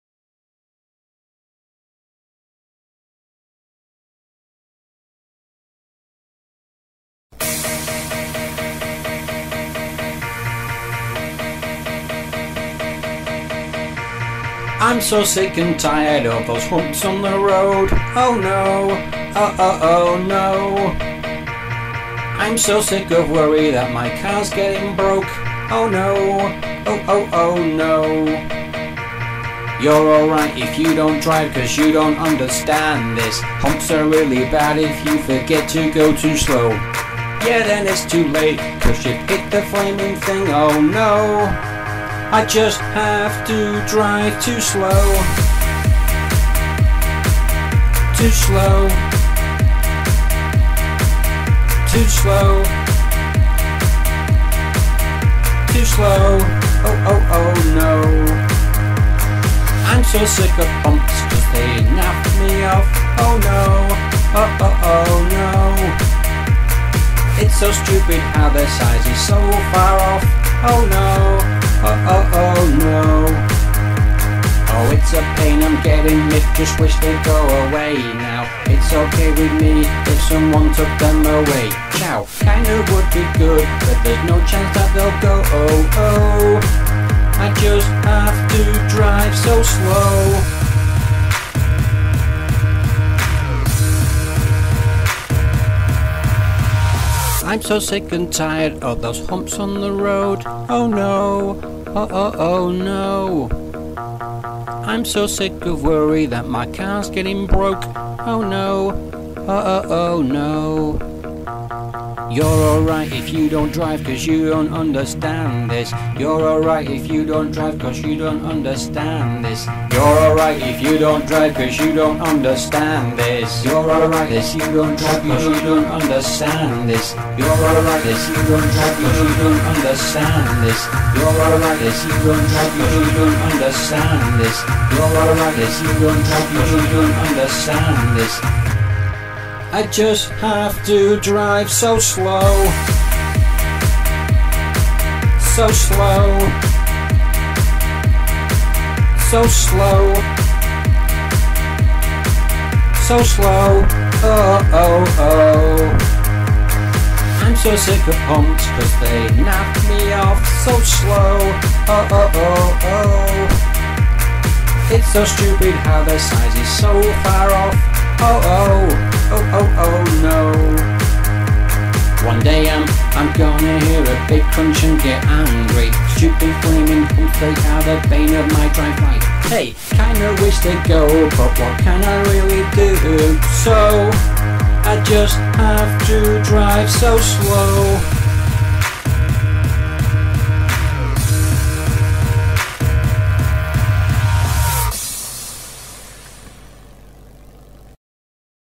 Stupid Road Humps - Naff me off!
Those flaming stupid road humps naff me off they do!